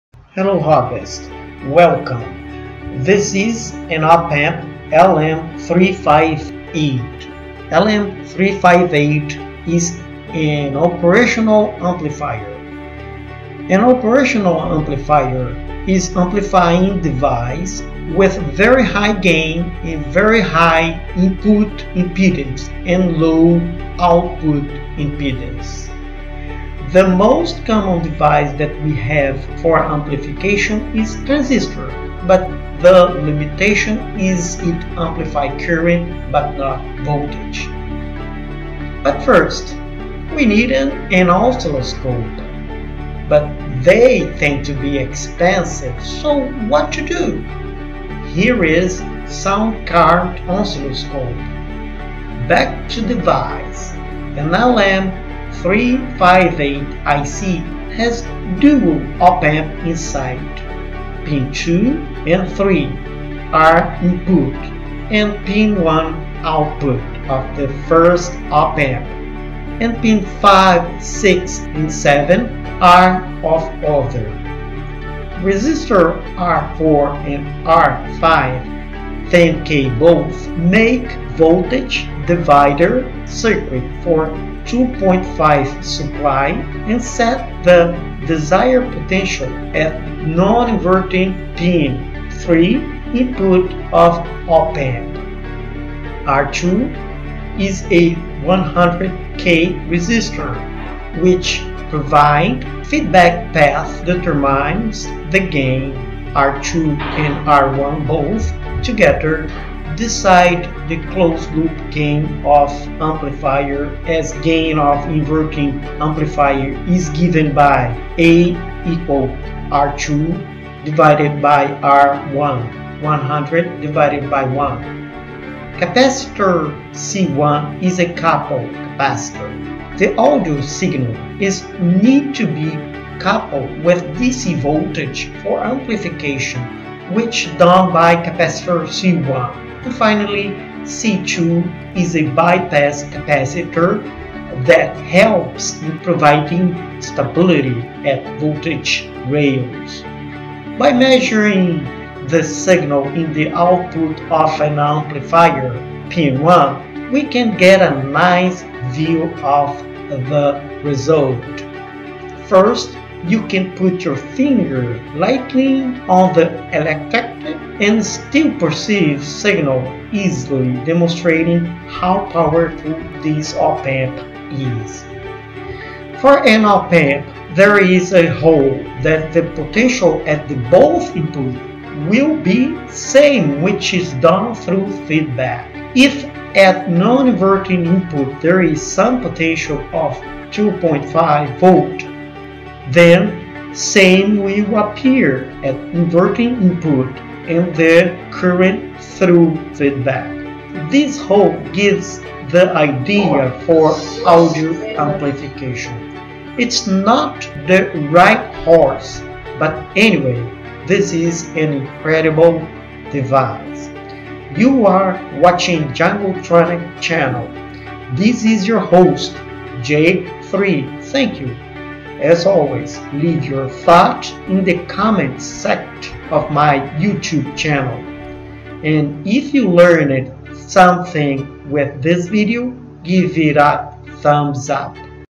LM358 Opamp Electret Amplifier Circuit - basicTronics - 01
This video  gives you the whole  idea about  audio amplification. LM358 is a operational amplifier.